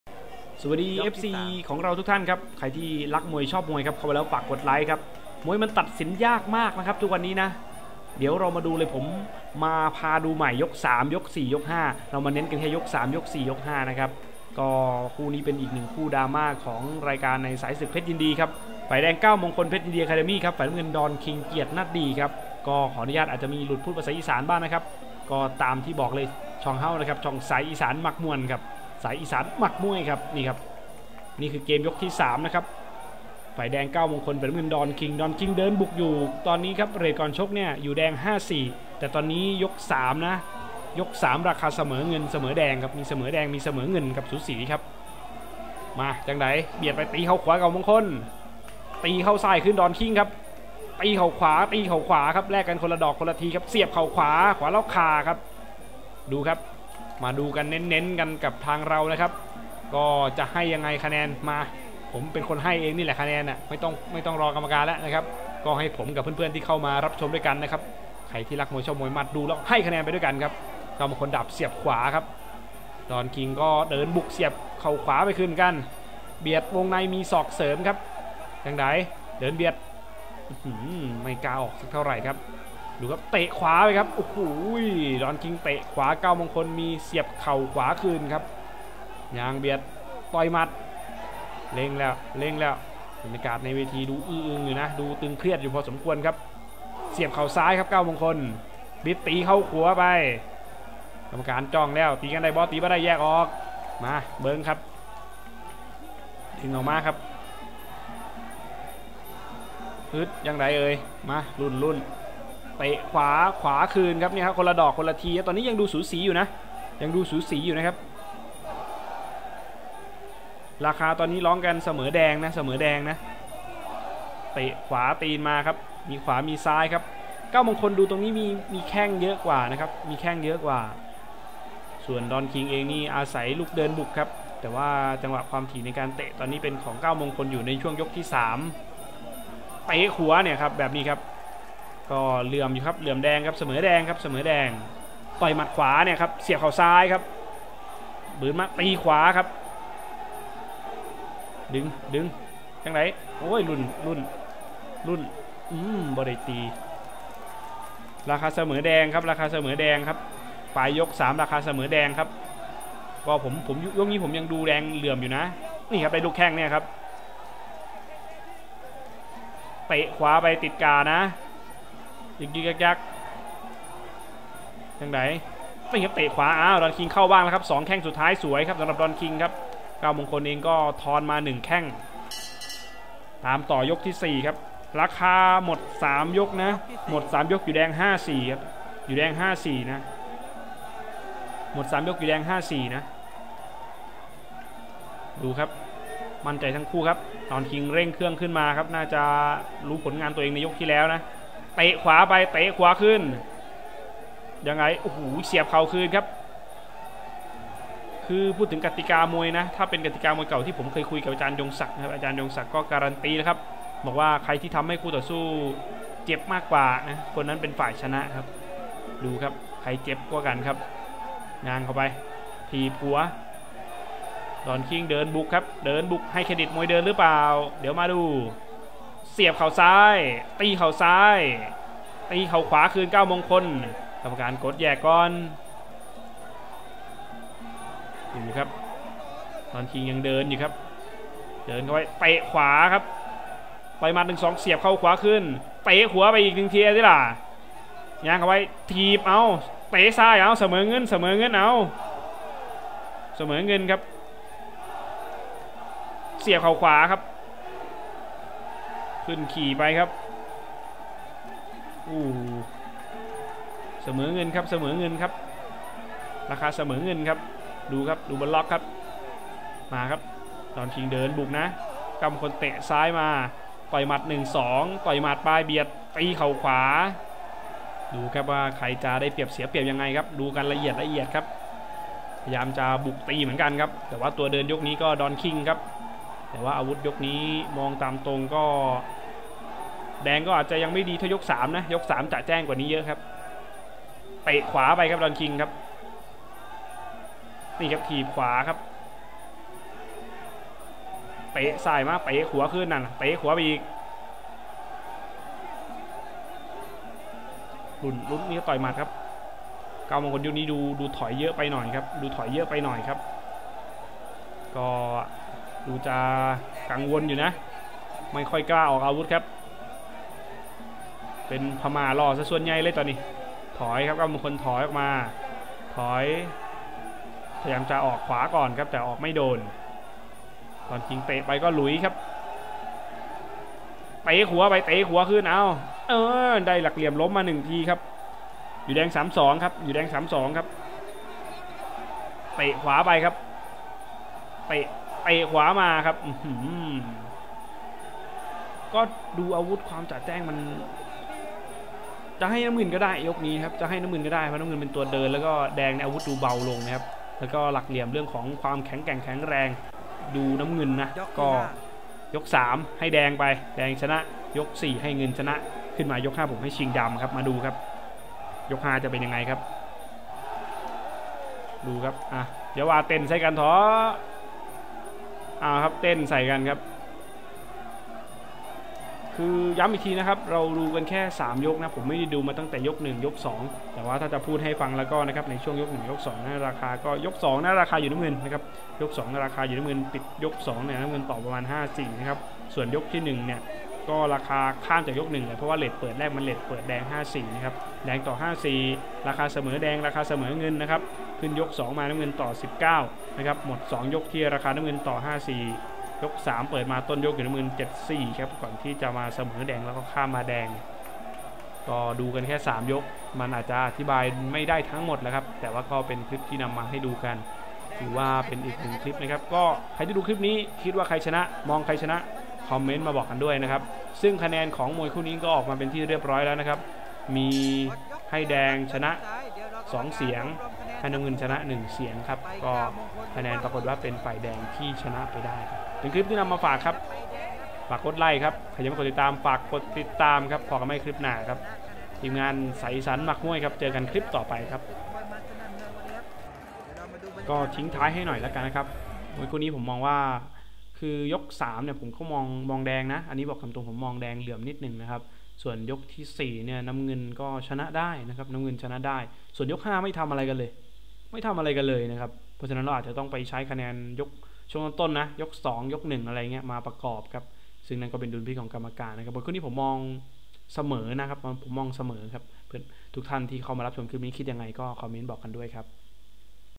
สุดเบียด!! เก้ามงคล เพชรยินดีอะคาเดมี่ สู้กับ ดอนคิงส์ เกียรตินะดีย์ ศึก​มวยมัน​ส์​สนั่น​เมือง
คู่4.​ ​เก้า​มงคล​ เพชร​ยินดี​อะคาเด​มี่​(แดง)​ชนะ​คะแนน​ ดอน​คิงส์​ เกียรติ​นะ​ดี​ คู่เอก​"ศึก​มวยมัน​ส์​สนั่น​เมือง" (30ส.ค.65)เวทีมวย​รังสิต